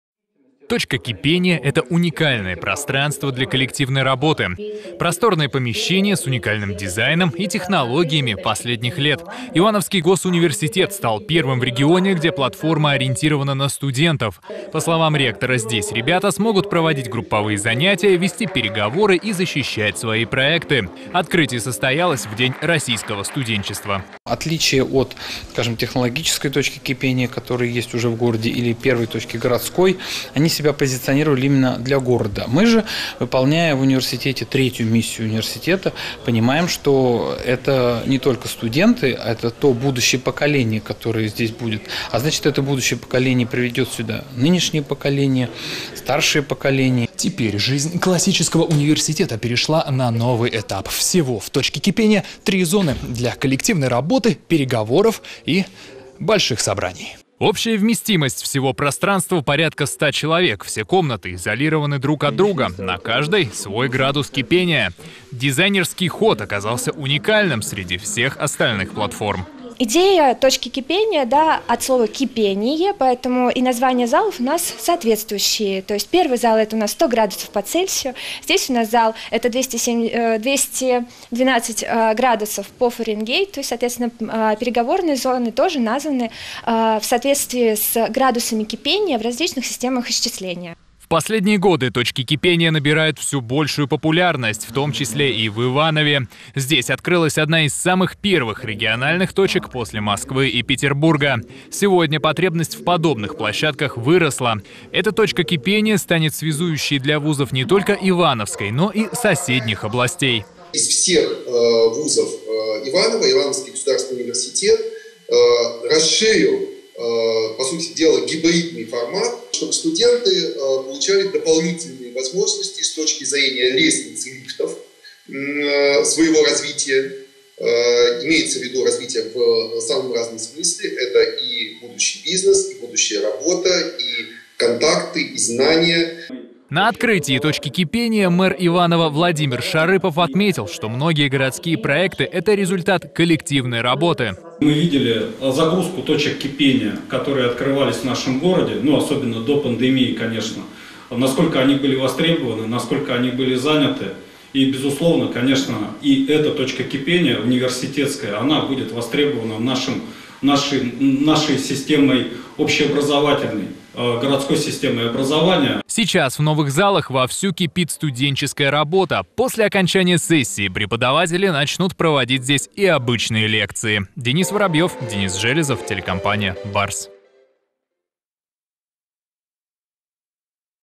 Открылась «Точка кипения – ИвГУ»
25 января, в День российского студенчества, произошло поистине знаменательное событие – торжественное открытие «Точки кипения – ИвГУ» с участием представителей федеральных и региональных органов власти. 3-я в регионе и 141-я в стране – но первая в области «вузовская», связанная и с расширением самого формата высшего образования, и с открывающимися новыми возможностями коллаборации в составе самых различных сообществ и объединений.